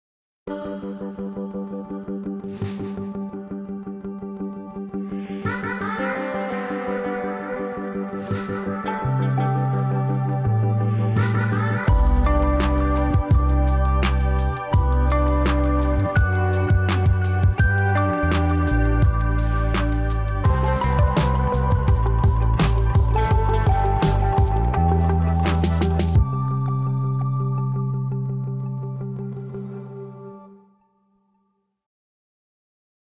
Wings of Fire Photo Movie with relaxing music
These are photos of the Wings of Fire book series by Tui .T Sutherland. Hope you guys enjoy!